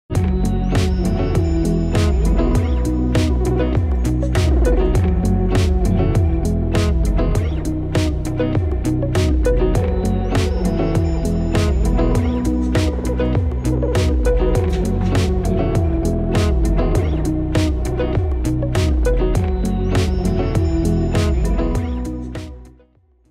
Tir à l'arc : comment retirer vos flèches de la cible
L'utilisation d'un tire flèche vous facilitera l'extraction de vos flèches.